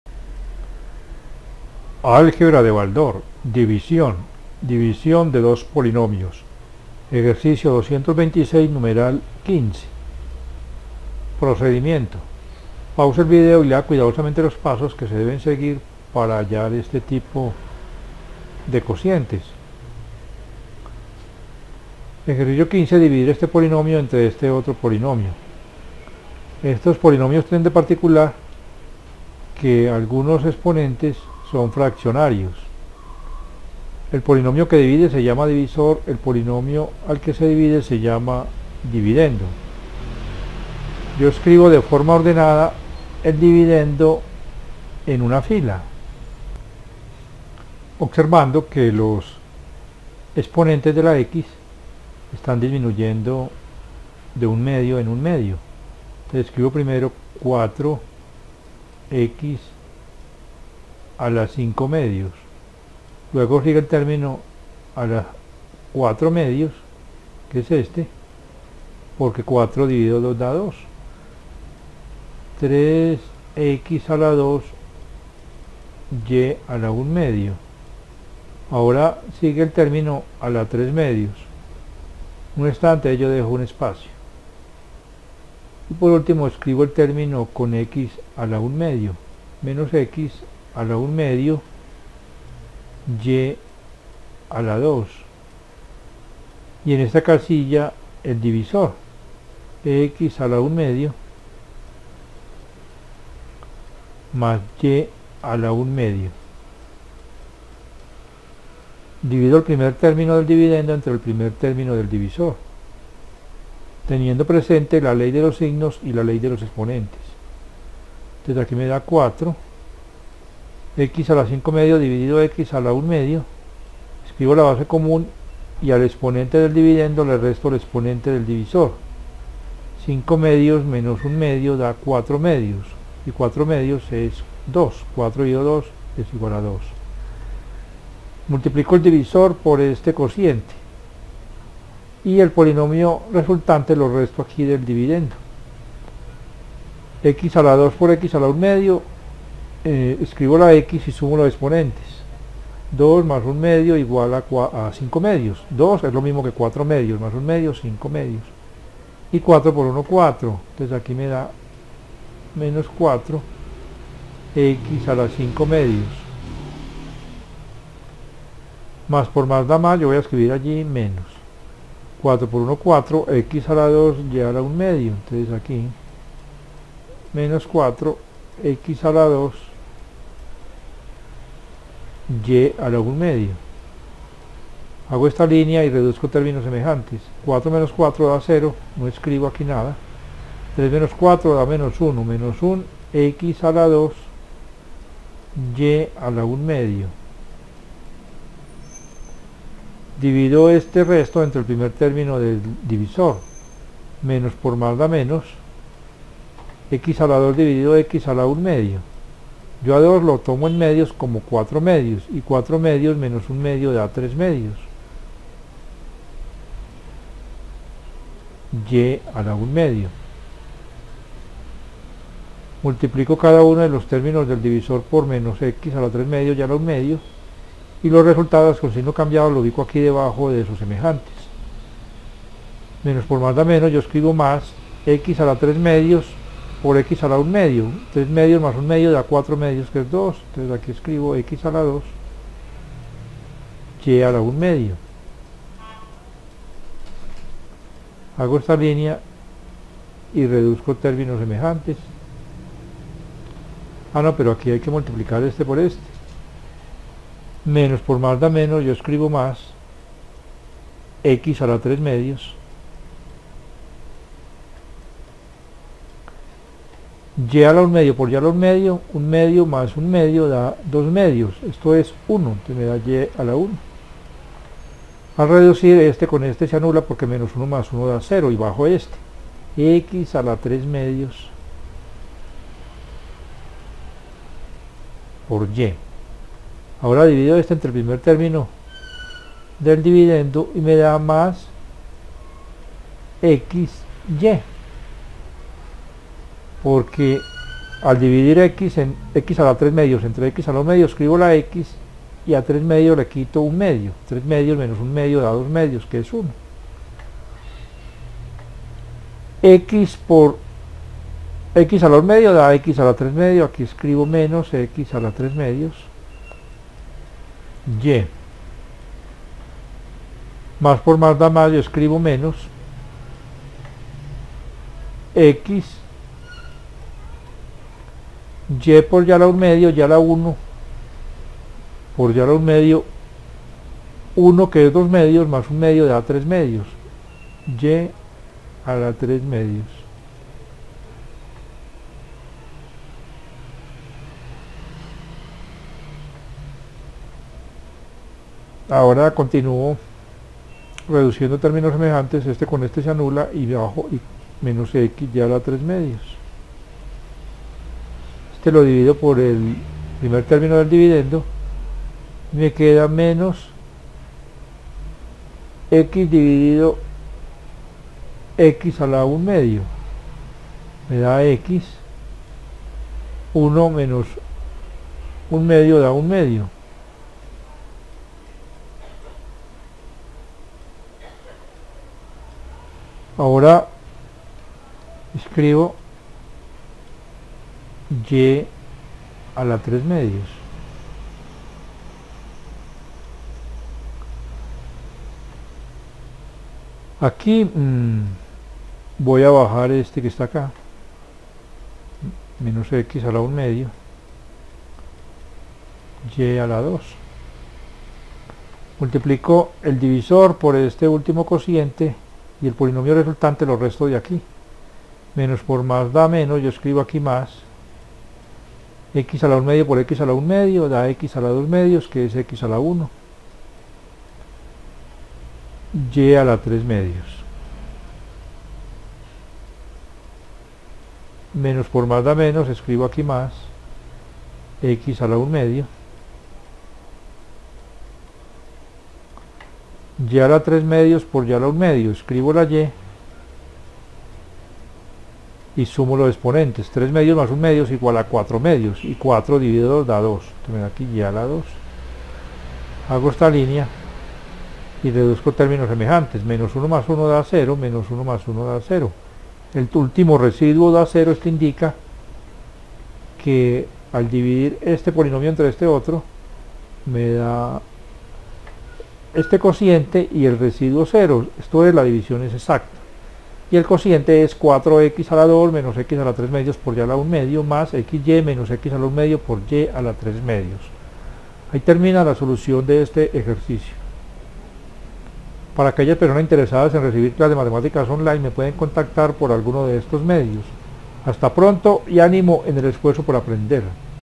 Baldor 226 15
Teoría de los exponentes. División de polinomios con exponentes fraccionarios.